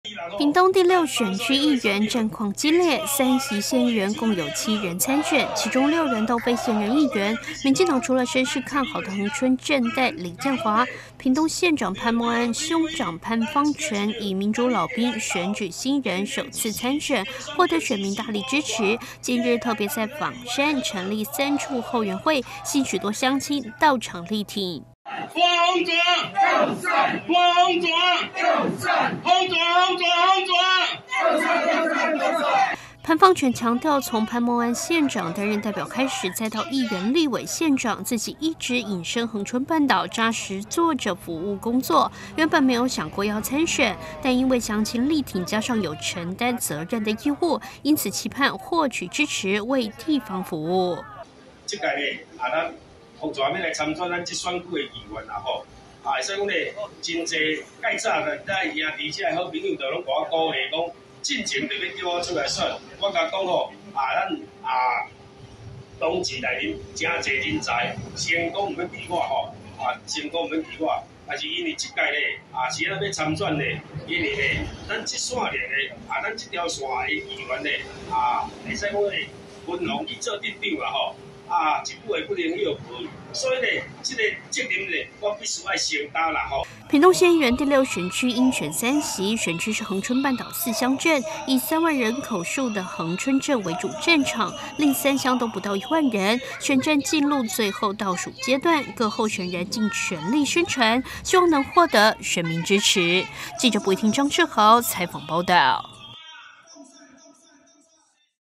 枋山鄉/選舉新聞 111 1020 潘芳泉枋山成立後援會 支持者到場力挺
屏東第六選區議員戰況激烈，其中屏東縣長潘孟安的兄長 潘芳泉，特別在 加祿、枋山及楓港成立三處後援會，除了眾多支持者前往加油打氣，屏東縣長候選人-立委周春米以及現任縣長潘孟安也特別現身力挺。